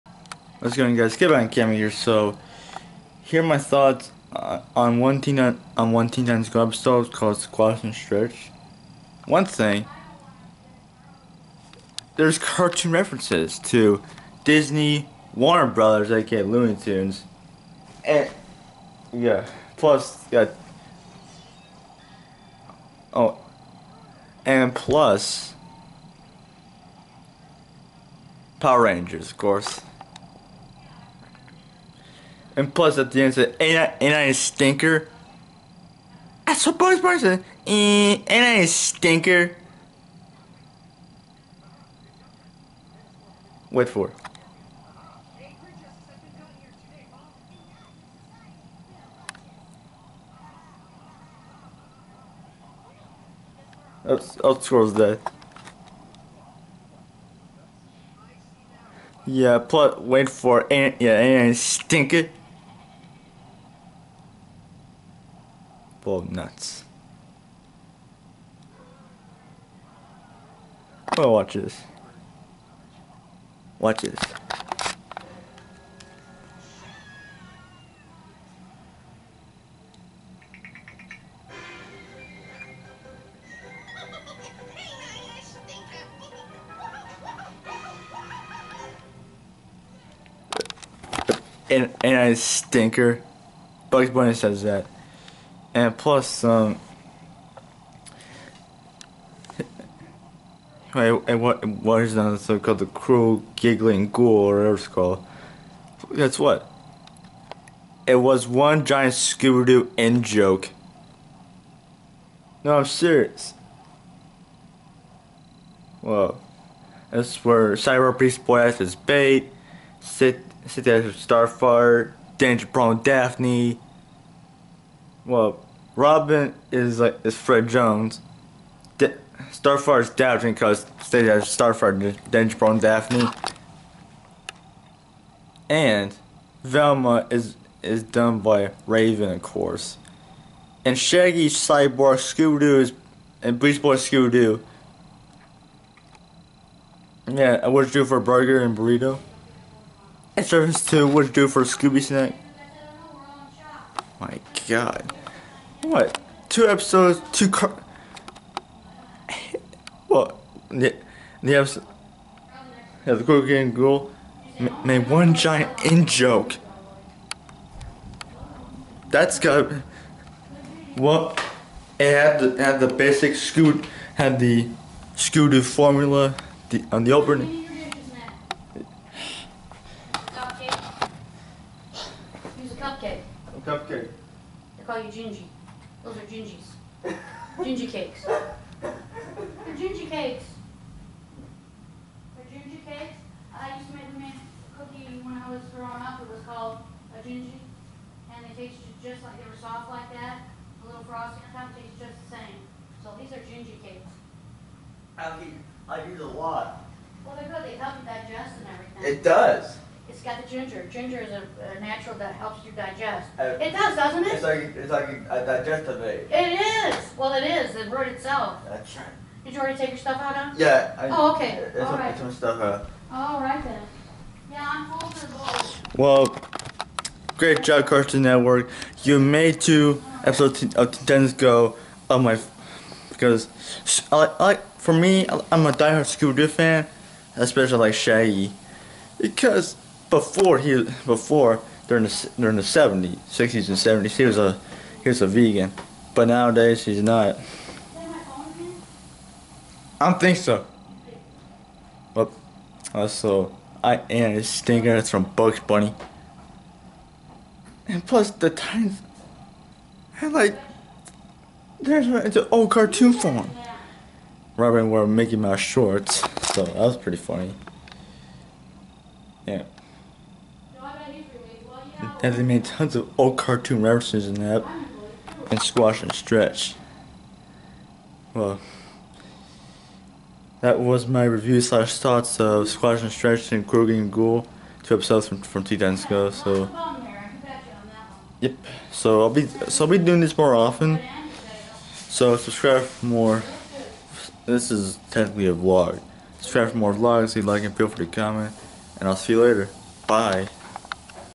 MY THOUGHTS ON TEEN TITANS GO!
Vlog #141- These are my thoughts on two episodes Teen Titans Go! One of them is called Squash & Stretch which makes old cartoon references an the other episode makes on e giant Scooby Doo in joke.